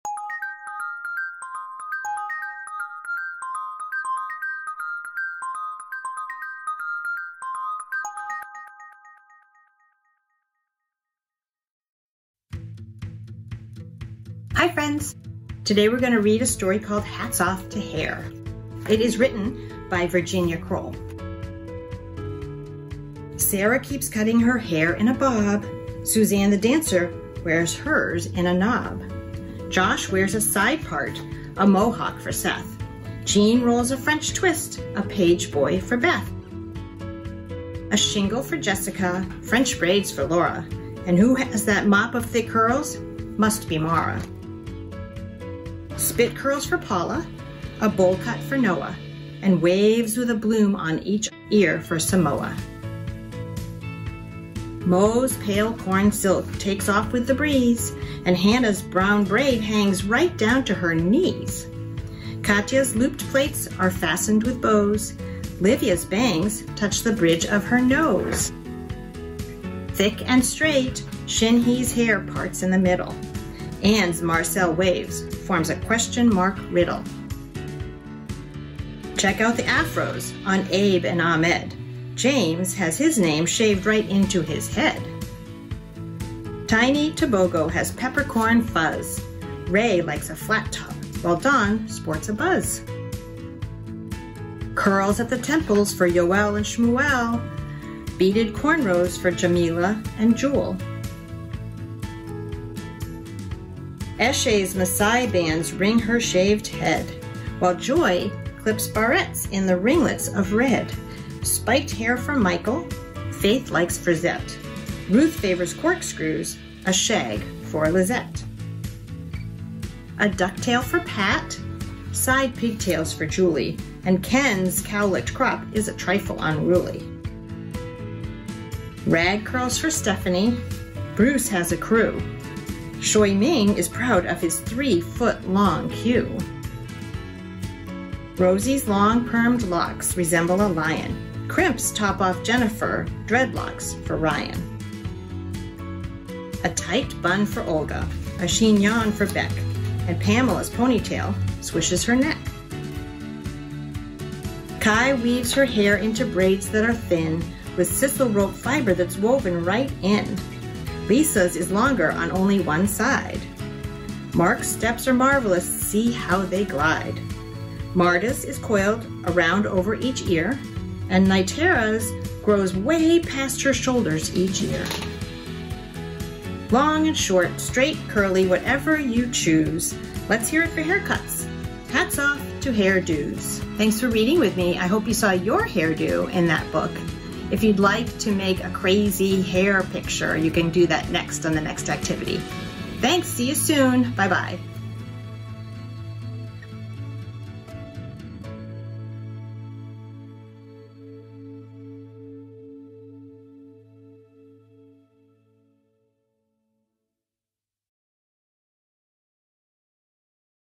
Reading of "Hats Off To Hair" by Virginia Kroll
Hats Off To Hair! by Virginia Kroll, a wonderful story for celebrating the beauty in diversity, read aloud.

From the Go2sLearnAtHome Activity Set All About Me 1.
Learning Domain: Literacy, Social/Personal, Social Studies

Readiness Skills: book awareness, print awareness, self awareness, people similarities & differences

Sign up for Go2sLearnAtHome for access to FREE preschool activity sets, created by veteran educators and based on Creative Curriculum(tm) themes